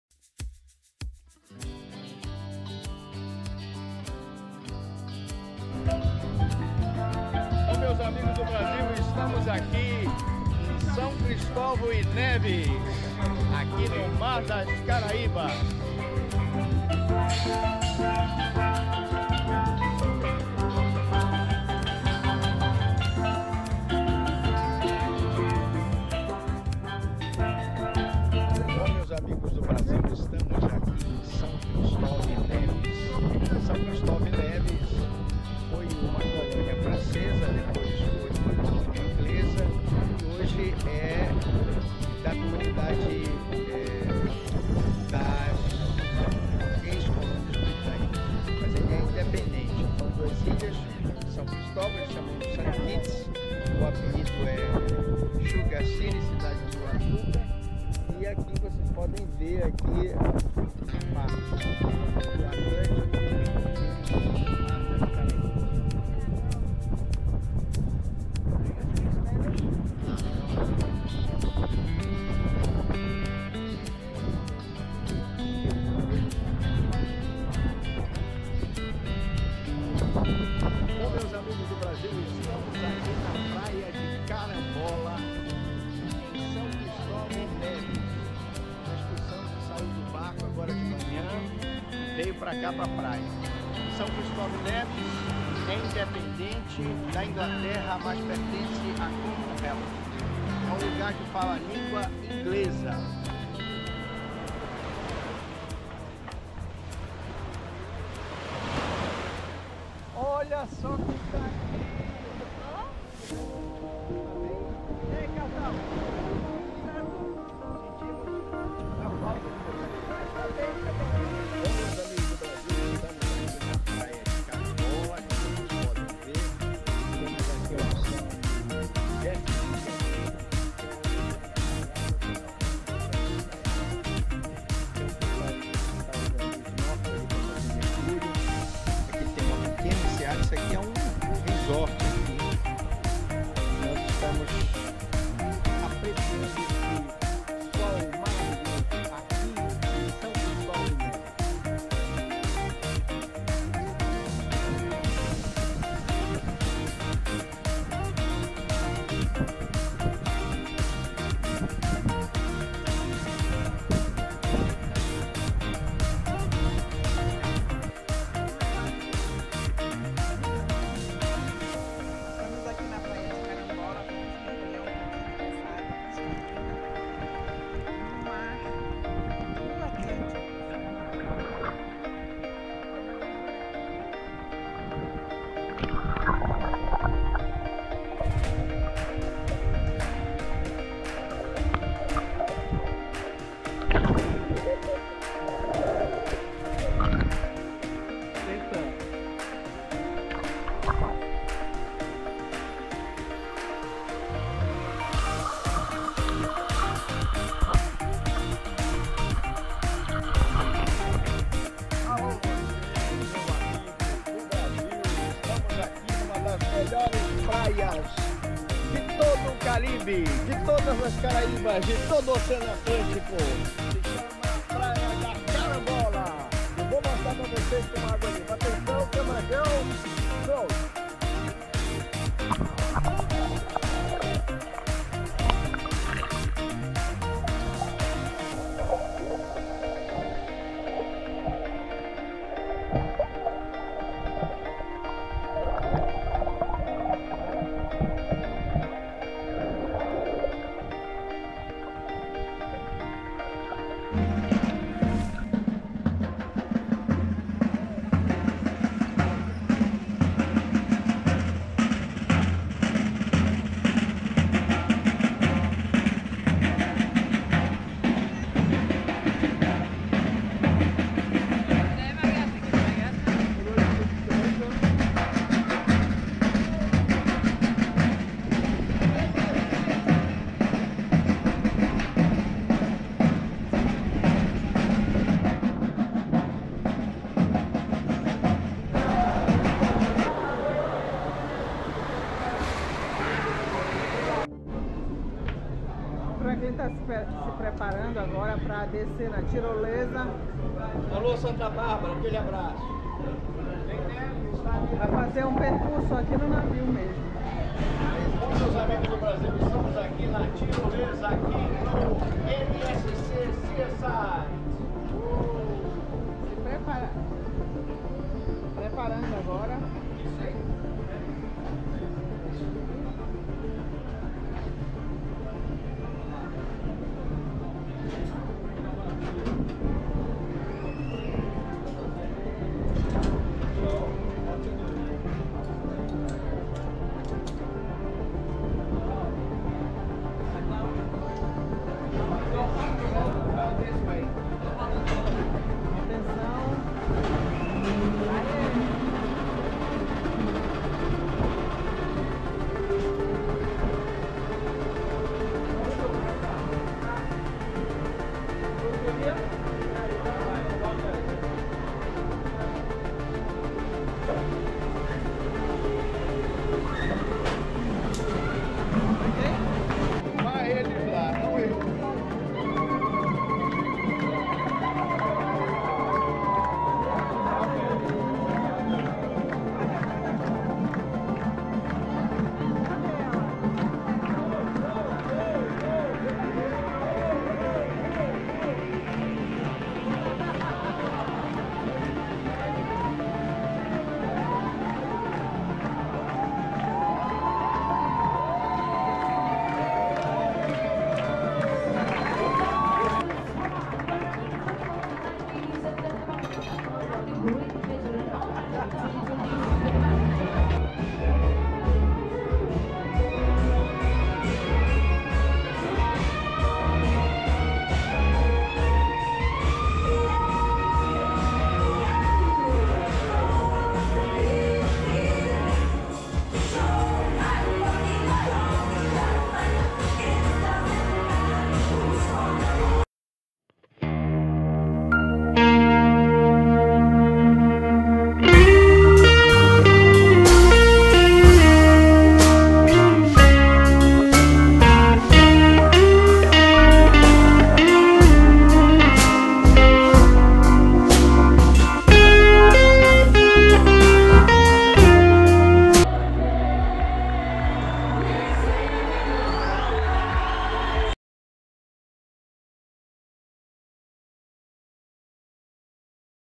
07. São Cristovão e Neves 2024
São Cristóvão e Neves foi dos primeiros arquipélagos das Caraíbas a ser povoado por europeus. A ilha de São Cristóvão recebeu quer a primeira colónia britânica, quer a primeira colónia francesa nas Caraíbas, e como tal é por vezes referida como "a colónia-mãe das Índias Ocidentais".